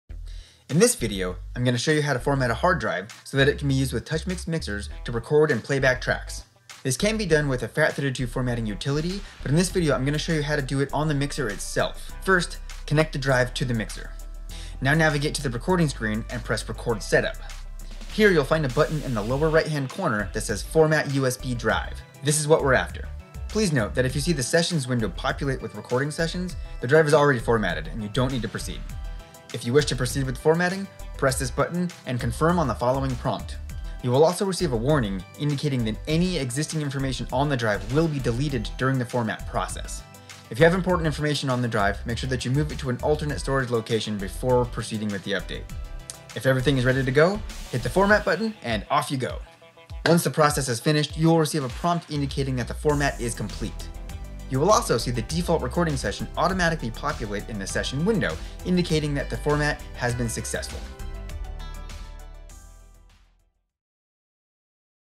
Formatting a USB drive on a TouchMix - Quick Tip
Learn how to format a USB drive to FAT32 using the TouchMix onboard formatting utility.